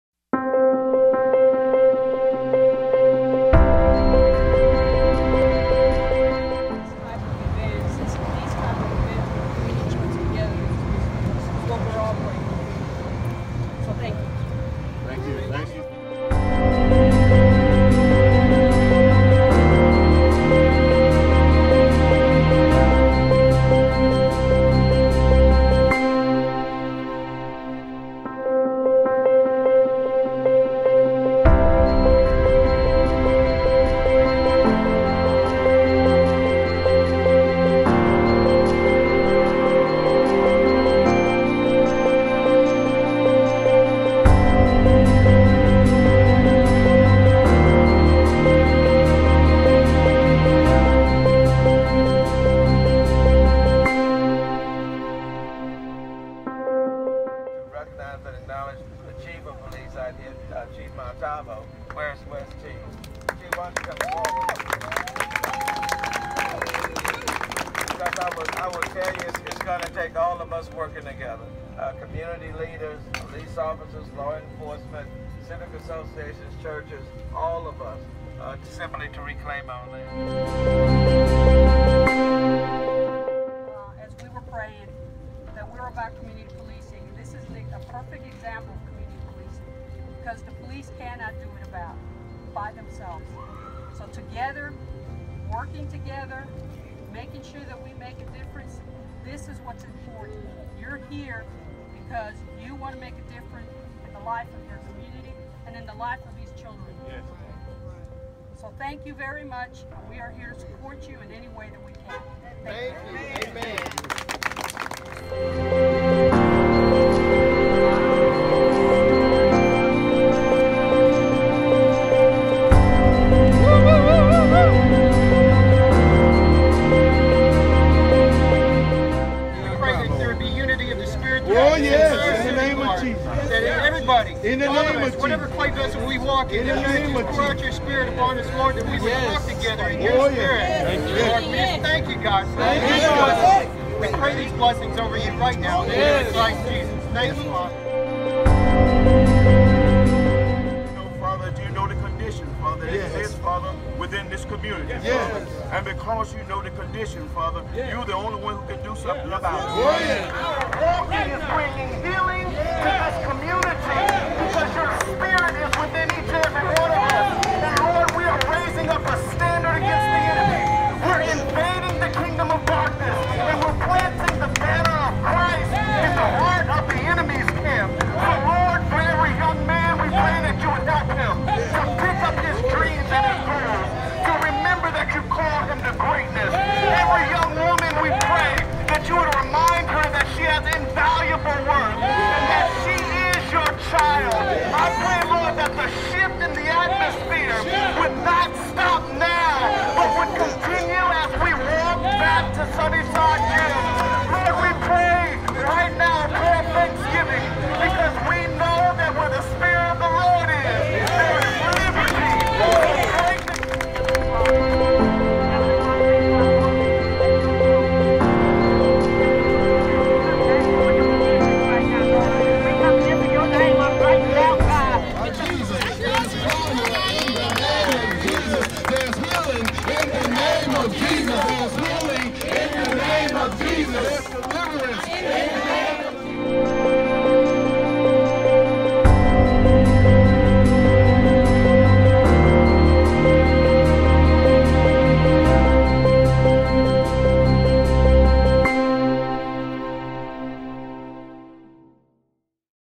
Sunnyside Walk for Peace
Pastor Calvin and Lady Helen Miller organized the Sunnyside Walk for Peace. Several churches in the community, including the World Harvest Outreach SDA Church marched and prayed for peace in the streets of Sunnyside on March 26, 2016